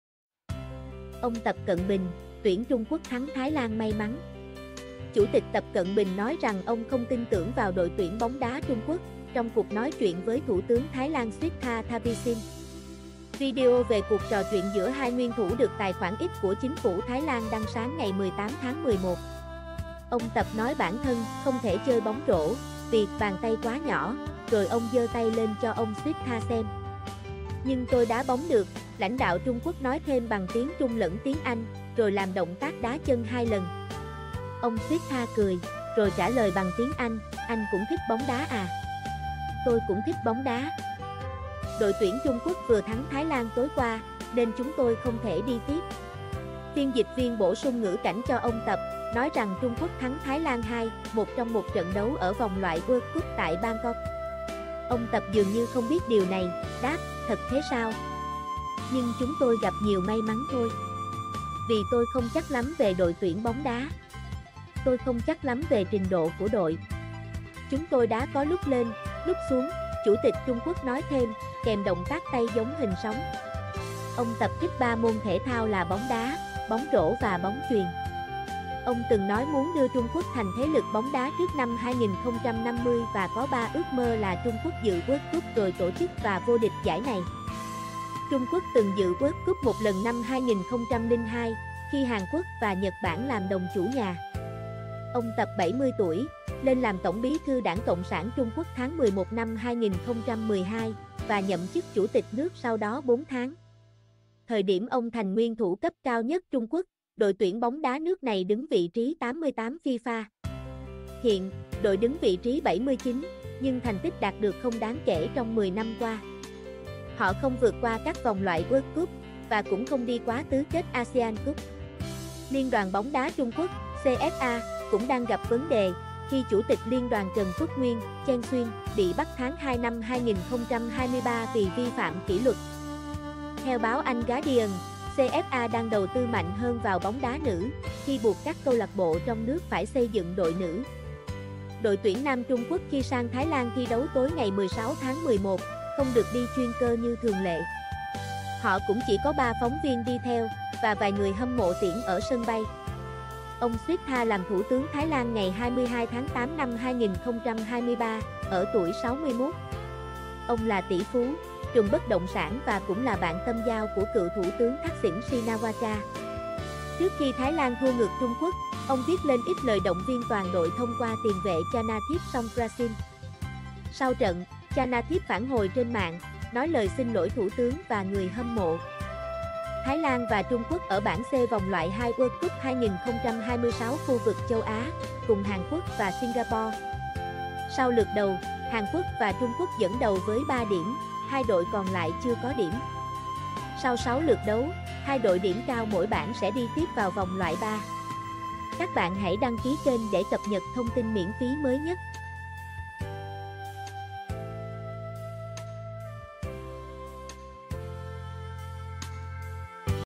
Ông Tập Cận Bình: 'Tuyển Trung Quốc thắng Thái Lan may mắn'
Chủ tịch Tập Cận Bình nói rằng ông không tin tưởng vào đội tuyển bóng đá Trung Quốc, trong cuộc nói chuyện với Thủ tướng Thái Lan Srettha Thavisin.

Video về cuộc trò chuyện giữa hai nguyên thủ được tài khoản X của chính phủ Thái Lan đăng sáng 18/11. Ông Tập nói bản thân "không thể chơi bóng rổ", vì "bàn tay quá nhỏ", rồi ông giơ tay lên cho ông Srettha xem.

"Nhưng tôi đá bóng được", lãnh đạo Trung Quốc nói thêm bằng tiếng Trung lẫn tiếng Anh, rồi làm động tác đá chân hai lần.

Ông Srettha cười, rồi trả lời bằng tiếng Anh: "Anh cũng thích bóng đá à? Tôi cũng thích bóng đá. Đội tuyển Trung Quốc vừa thắng Thái Lan tối qua, nên chúng tôi không thể đi tiếp".

Phiên dịch viên bổ sung ngữ cảnh cho ông Tập, nói rằng Trung Quốc thắng Thái Lan 2-1 trong một trận đấu ở vòng loại World Cup tại Bangkok. Ông Tập dường như không biết điều này, đáp: "Thật thế sao?".

"Nhưng chúng tôi gặp nhiều may mắn thôi. Vì tôi không chắc lắm về đội tuyển bóng đá. Tôi không chắc lắm về trình độ của đội. Chúng tôi đá có lúc lên, lúc xuống", Chủ tịch Trung Quốc nói thêm, kèm động tác tay giống hình sóng.

Ông Tập thích ba môn thể thao là bóng đá, bóng rổ và bóng chuyền. Ông từng nói muốn đưa Trung Quốc thành thế lực bóng đá trước năm 2050 và có ba ước mơ là Trung Quốc dự World Cup rồi tổ chức và vô địch giải này. Trung Quốc từng dự World Cup một lần năm 2002, khi Hàn Quốc và Nhật Bản làm đồng chủ nhà.

Ông Tập 70 tuổi, lên làm Tổng bí thư đảng Cộng sản Trung Quốc tháng 11/2012, và nhậm chức Chủ tịch nước sau đó bốn tháng. Thời điểm ông thành nguyên thủ cấp cao nhất Trung Quốc, đội tuyển bóng đá nước này đứng vị trí 88 FIFA. Hiện, đội đứng vị trí 79, nhưng thành tích đạt được không đáng kể trong 10 năm qua. Họ không vượt qua các vòng loại World Cup, và cũng không đi quá tứ kết Asian Cup.

Liên đoàn Bóng đá Trung Quốc (CFA) cũng đang gặp vấn đề, khi Chủ tịch Liên đoàn Trần Tuất Nguyên (Chen Xuyuan) bị bắt tháng 2/2023 vì vi phạm kỷ luật. Theo báo Anh Guardian, CFA đang đầu tư mạnh hơn vào bóng đá nữ, khi buộc các CLB trong nước phải xây dựng đội nữ. Đội tuyển nam Trung Quốc khi sang Thái Lan thi đấu tối 16/11, không được đi chuyên cơ như thường lệ. Họ cũng chỉ có ba phóng viên đi theo, và vài người hâm mộ tiễn ở sân bay.

Ông Srettha làm Thủ tướng Thái Lan ngày 22/8/2023, ở tuổi 61. Ông là tỷ phú, trùm bất động sản và cũng là bạn tâm giao của cựu thủ tướng Thaksin Shinawatra. Trước khi Thái Lan thua ngược Trung Quốc, ông viết lên X lời động viên toàn đội thông qua tiền vệ Chanathip Songkrasin. Sau trận, Chanathip phản hồi trên mạng, nói lời xin lỗi Thủ tướng và người hâm mộ.

Thái Lan và Trung Quốc ở bảng C vòng loại hai World Cup 2026 khu vực châu Á, cùng Hàn Quốc và Singapore. Sau lượt đầu, Hàn Quốc và Trung Quốc dẫn đầu với ba điểm, hai đội còn lại chưa có điểm. Sau sáu lượt đấu, hai đội điểm cao mỗi bảng sẽ đi tiếp vào vòng loại ba.